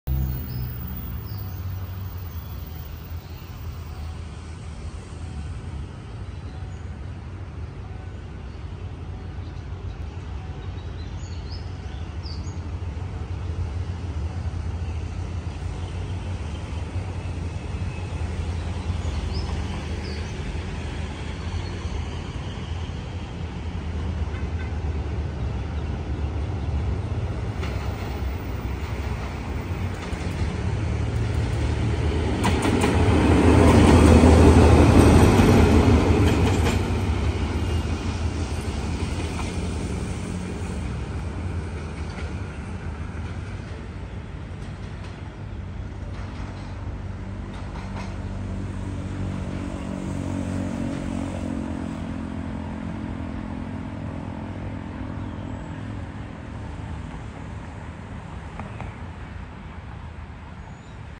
Rotfaithai Ep.1324 ฮิตาชิ 4501 ผู้นำของรุ่น เดินตัวเปล่าผ่านสามเสน ไปทำขบวน 171 ที่หัวลำโพง
Train : Locomotive Empty Train
Locomotive : Hitachi 4501
Date time : May 1, 2022, 11.02 AM.
Place : Samsen Station

ขบวนรถ : รถจักรดีเซลเดินตัวเปล่า
รถจักร : ฮิตาชิ 4501
วัน-เวลา : 1 พฤษภาคม 2565, 11.02 น.
สถานที่ : สถานีสามเสน